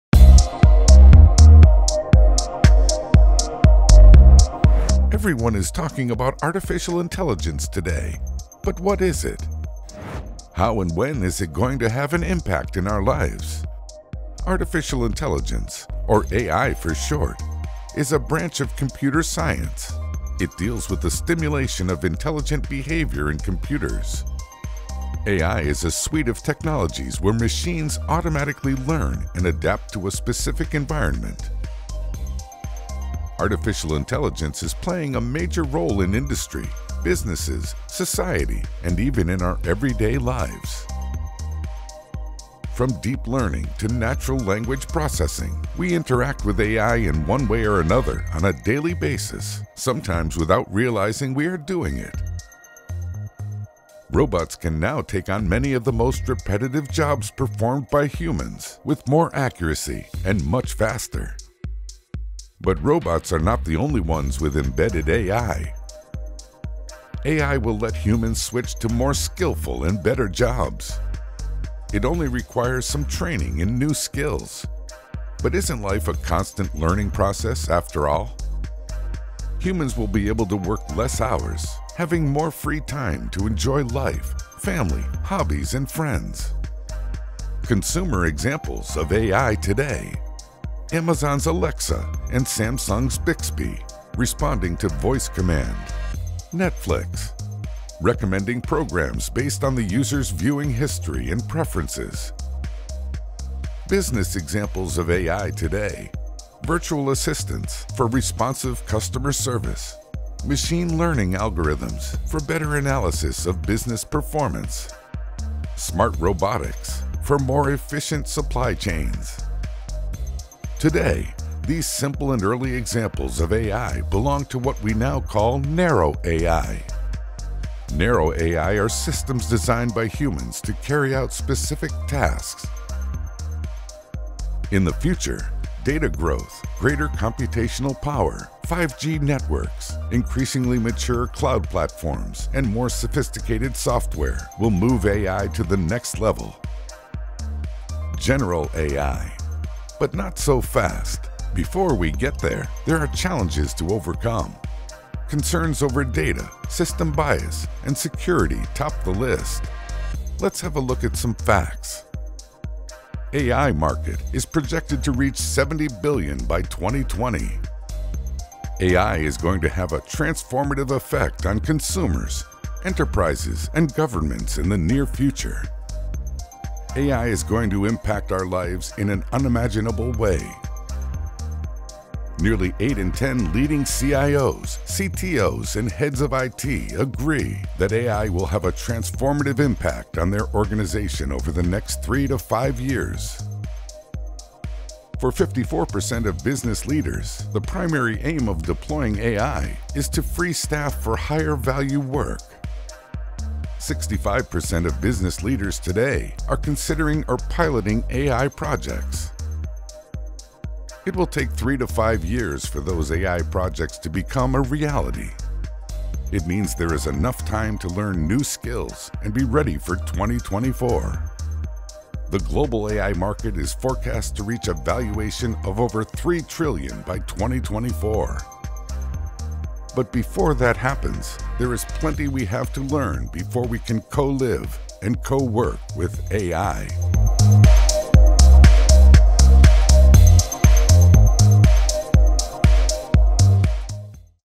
What is AI? How and when it is going to have an impact in our lives?
Everyone is talking about Artificial Intelligence today? But what is it? How and when it is going to have an impact in our lives?

Artificial Intelligence, or AI for short, is a branch of computer science. It deals with the stimulation of intelligent behavior in computers.

AI is a suite of technologies where machines automatically learn and adapt to a specific environment.

Artificial Intelligence is playing a major role in industry, businesses, society, and in our everyday lives.

From deep learning to natural language processing, we interact with AI in one way or another on a daily basis. Sometimes without realizing we are doing it.

Robots can now take on many of the most repetitive jobs performed by humans with more accuracy, and much faster. But robots are not the only ones with embedded AI.

AI will let humans switch to more skillful and better jobs.

It only requires some training in new skills. But isn't life a constant learning process after all?

Humans will be able to work less hours, having more free time to enjoy life, family, hobbies, and friends.